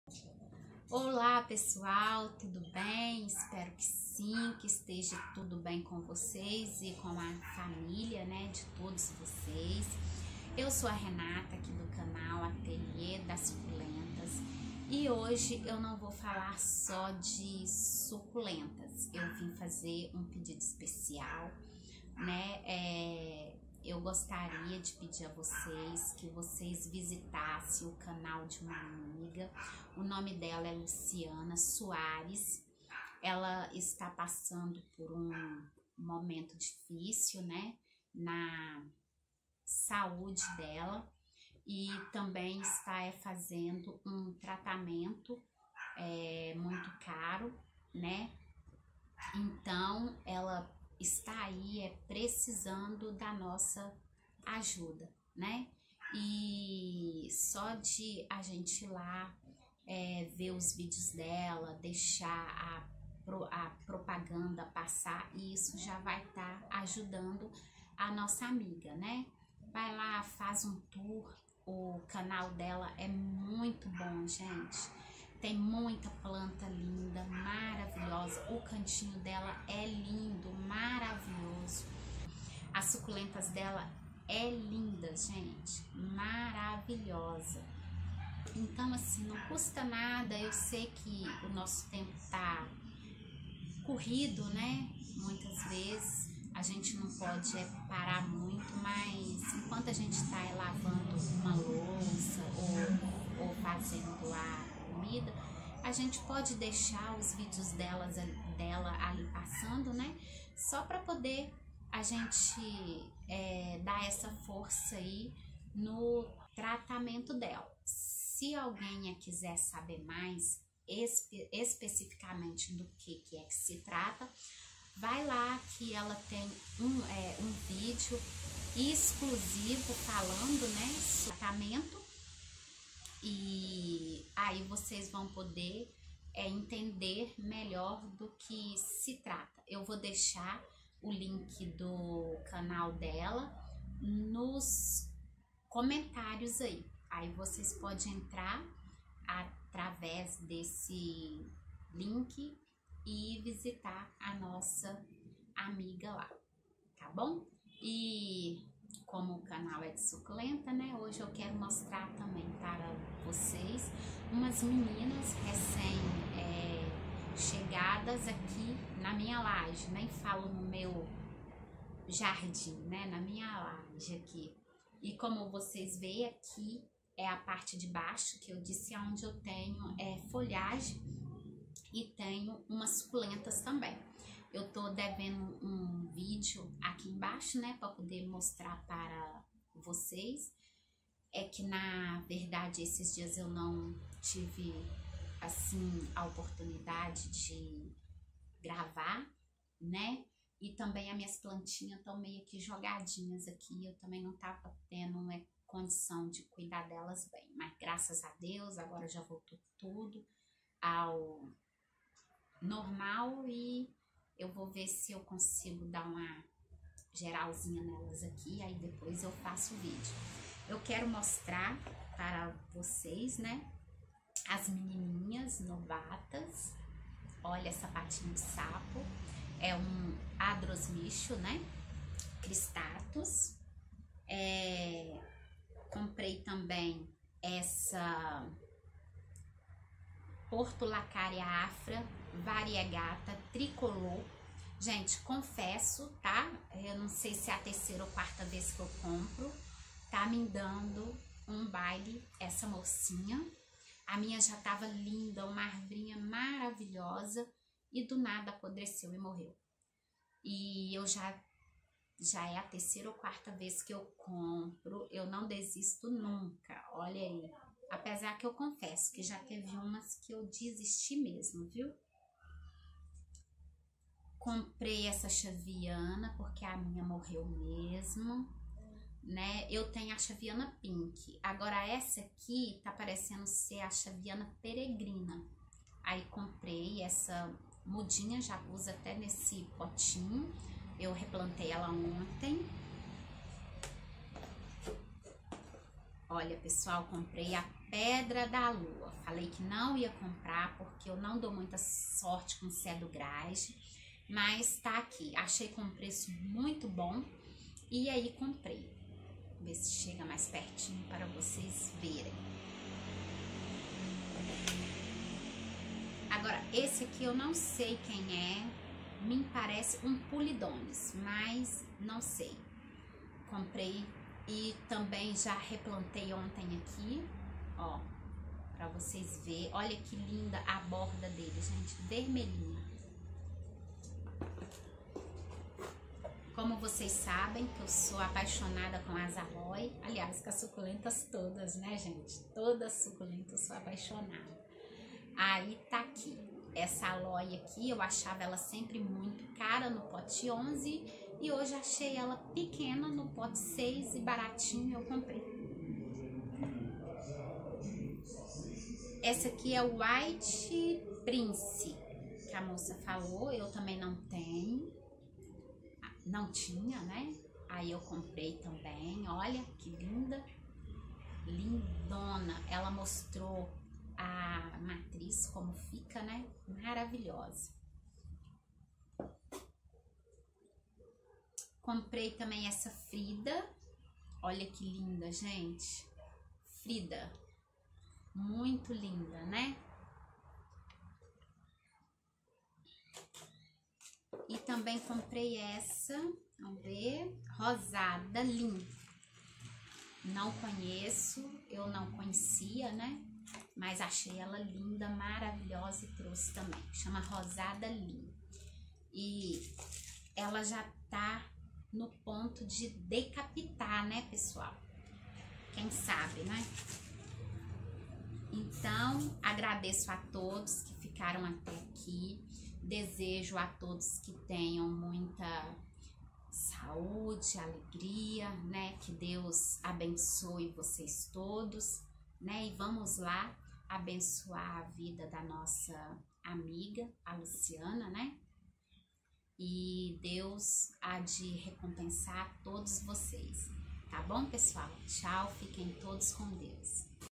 SUCULENTAS RECÉM CHEGADAS.
Nesse vídeo faço um pedido especial para vocês e mostro umas suculentas recém compradas 😘